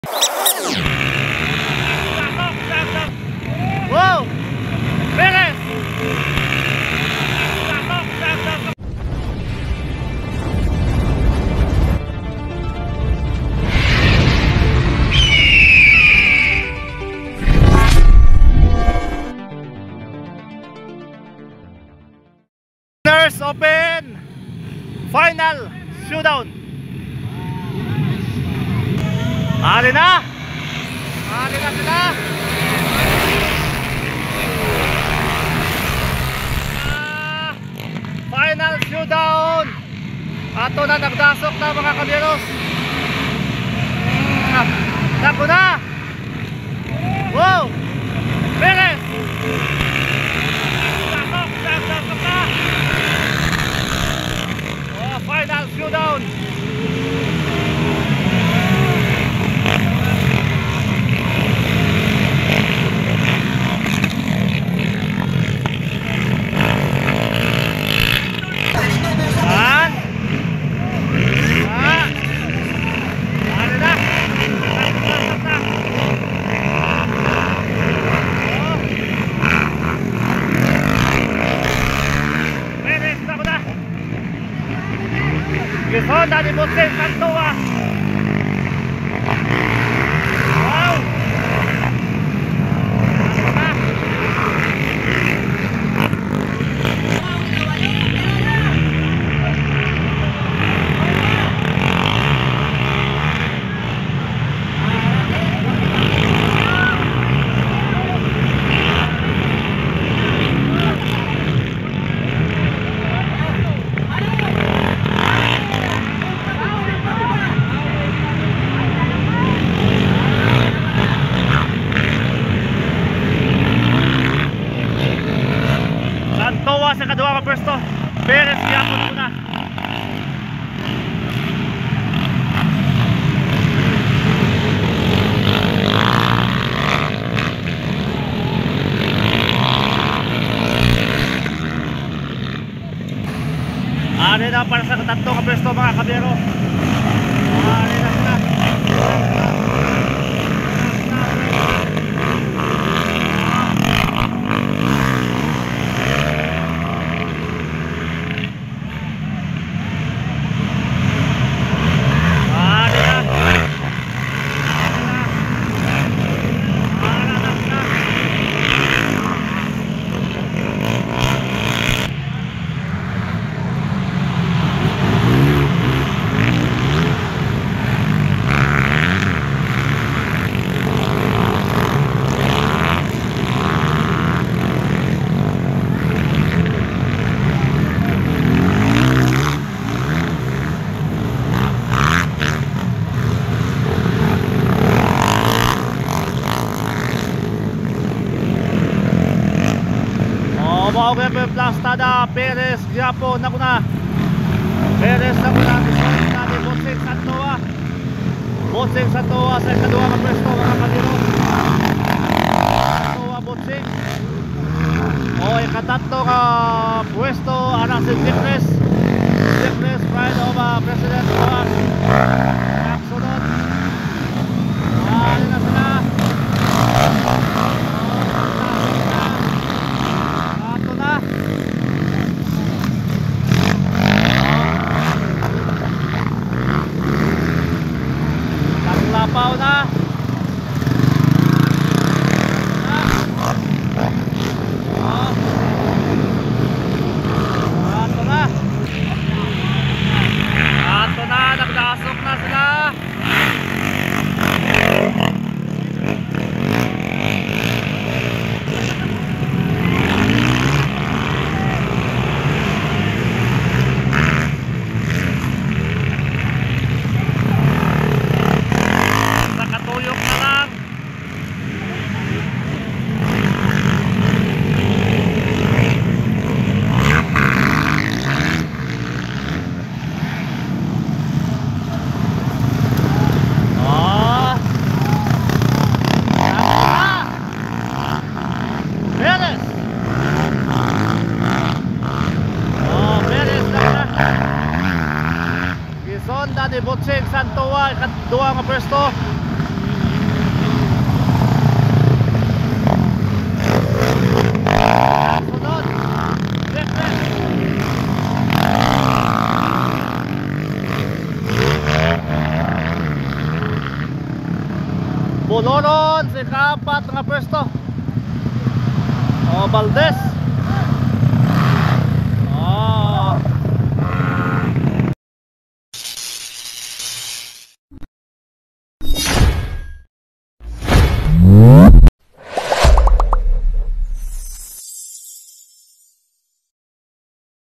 February 27, 2024
Bakbakan sa Camasi
motor trail competition
beginners Open
final showdown